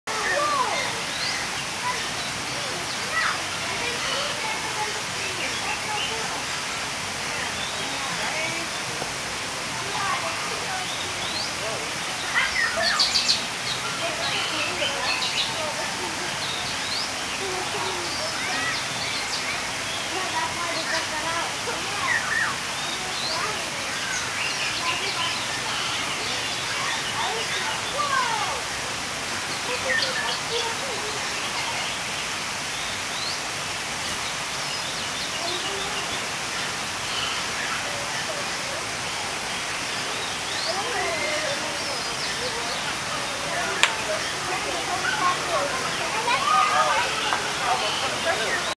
bower bird enjoying a banana above his bower house
he was very hungry while waitting for a bower girl